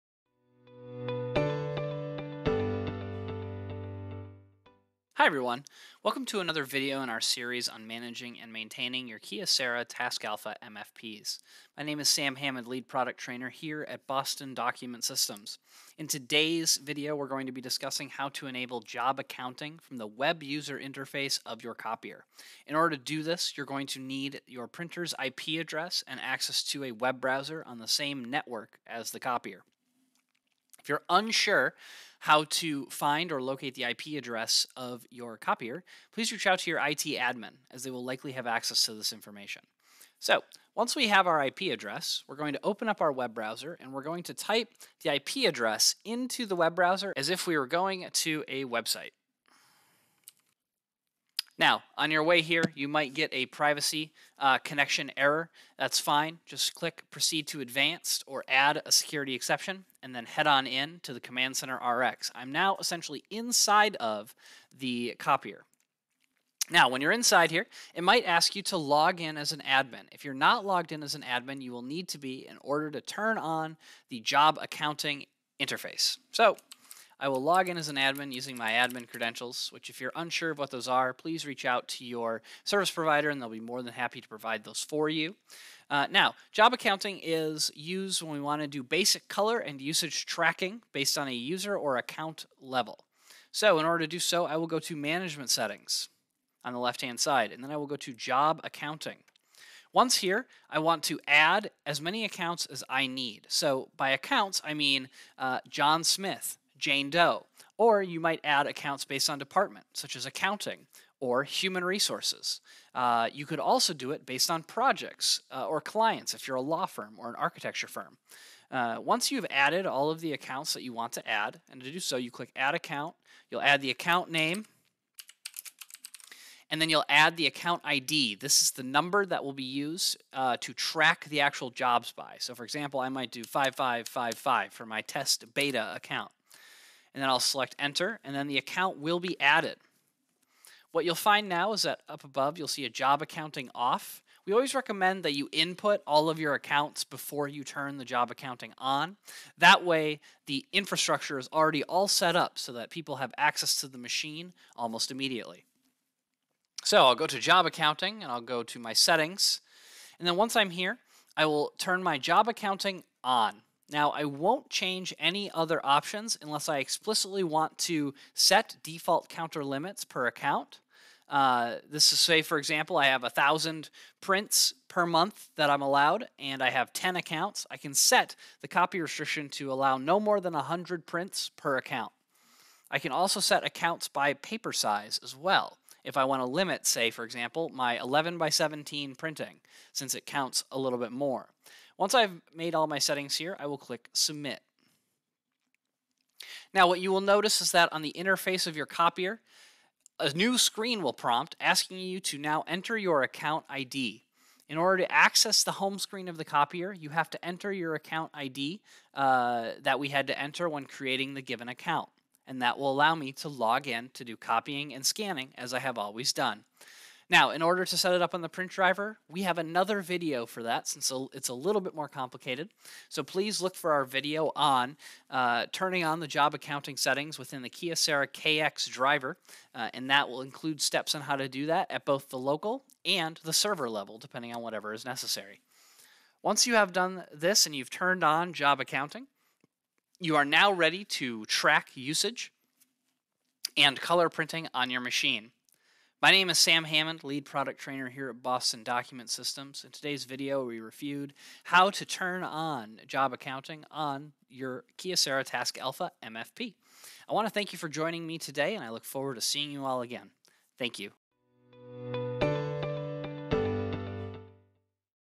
Turning on Job Accounting and adding Accounts on the Kyocera MFP
In this video, Sam walks you through how to enable Job Accounting and also how to add Job Accounts to the Kyocera MFP.